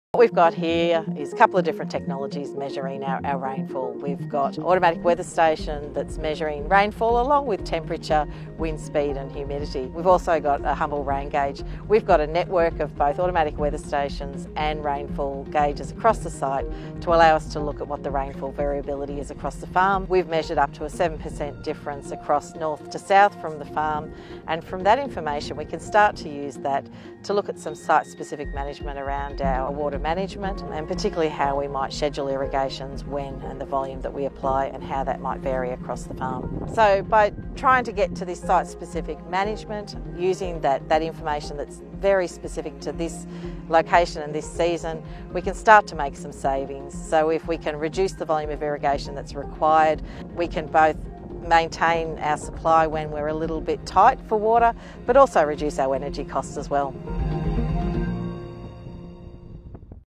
💧 Site specific water management saves $$
Justine Baillie and Michael Scobie are co-leads of the agtech adoption team at the University of Southern Queensland  - Australia and they're spearheading a program  to improve decision making on farm.

The project is providing tools and capacity for drought preparedness through the integration of digital technologies, data management, artificial intelligence and smart machinery including drones.